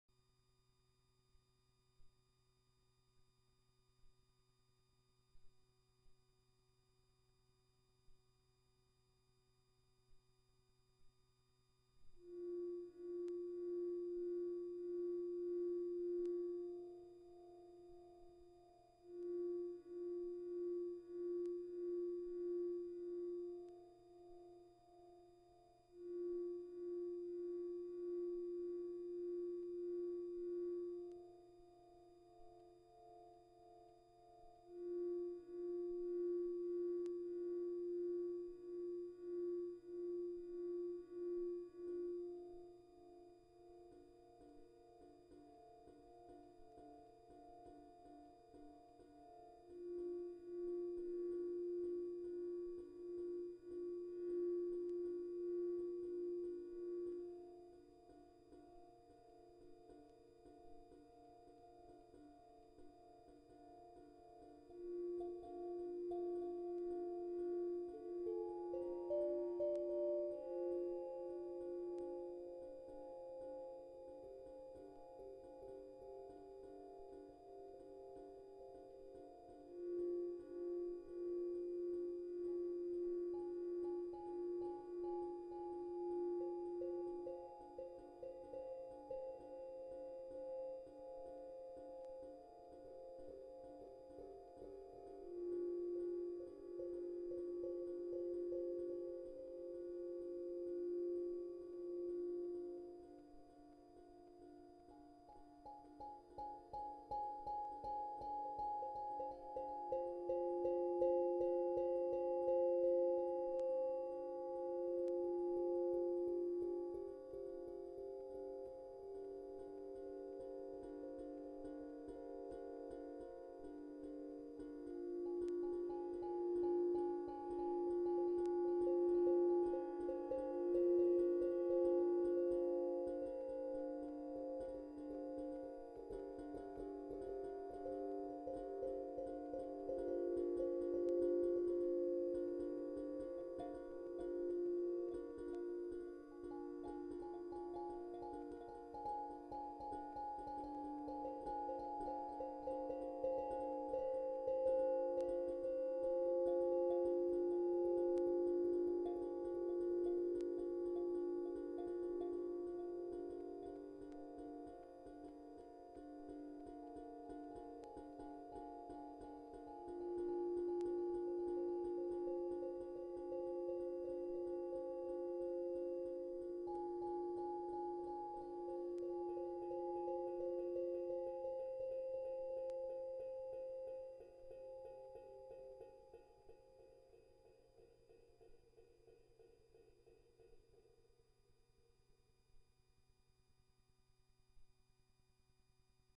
Jamuary 2022 day 1 - panda drum, norns and granchild
Granular reshuffling of panda drum through granchild on norns.

Easing into the new year.